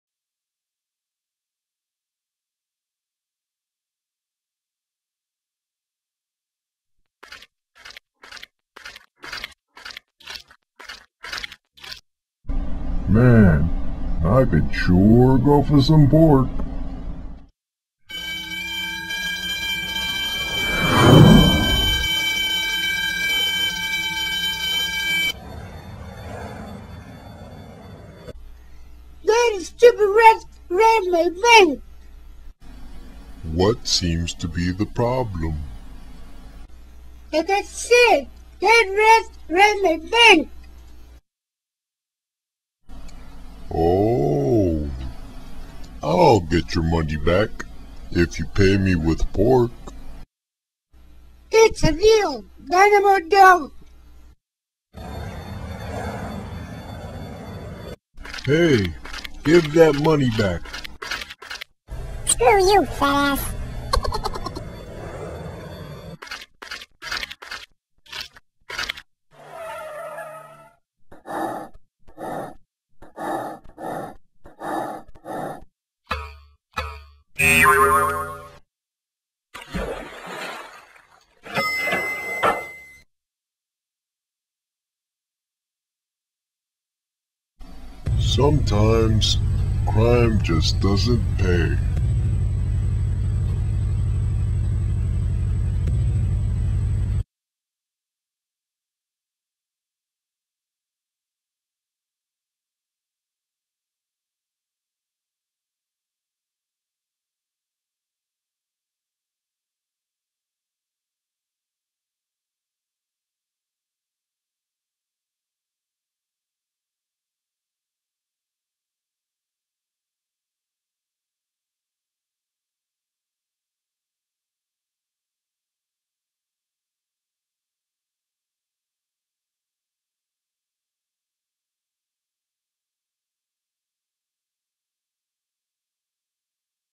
Roberto Moreno - Crime Doesn't Pay
(School Project) This was a my final project for 2D animation class based on characters I created for school assignments. Voice acting performed by my youngest brother and my dad.